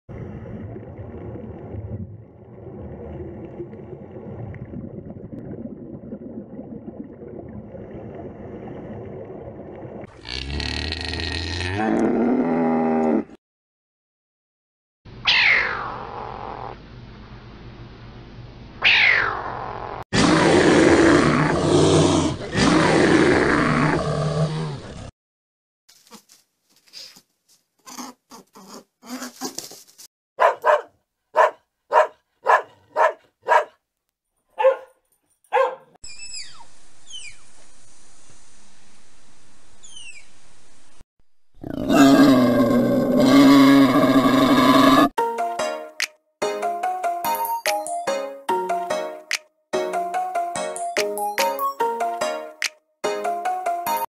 Animals sound octopus, megaloceros, caracal, bear, rabbit, dog, tapir, warthog part 2
Animals sound octopus, megaloceros, caracal, bear, rabbit,  dog, tapir, warthog.